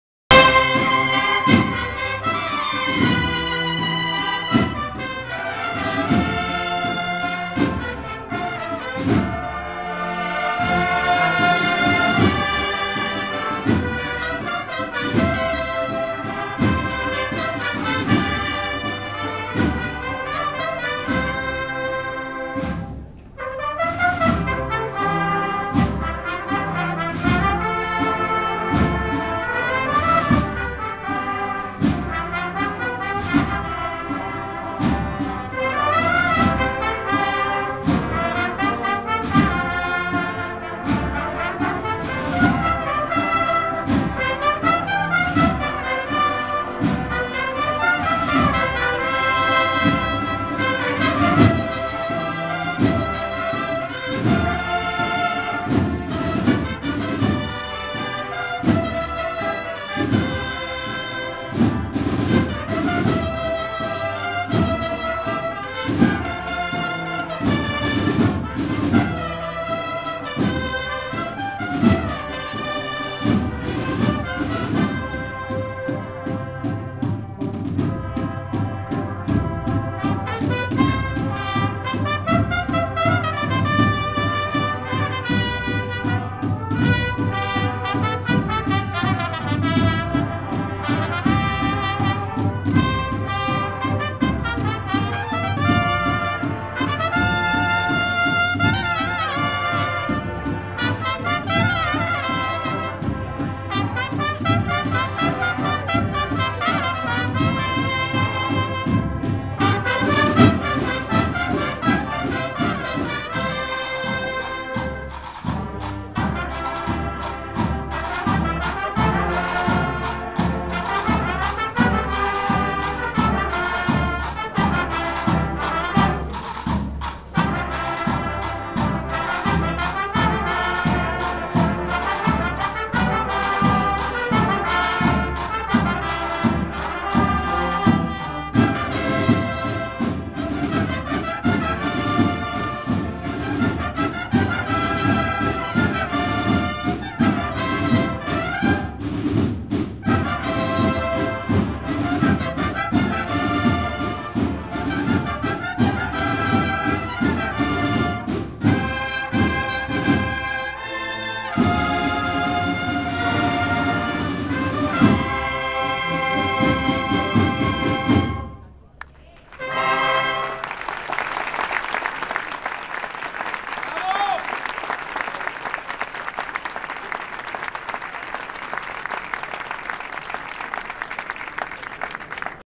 BANDA H. NTRA.SRA. DOLORS de OLIVA (3) I Trobada Bandas Cornetas i Tambores
La I Trobada de Bandas de Cornetas i Tambores de Oliva se realizo el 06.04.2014 a beneficio de Vedama (protectora de animales en Oliva). En este video la BANDA de la HERMANDAD NTRA.SRA.de los DOLORES de OLIVA toca otra de las piezas previstas para el evento.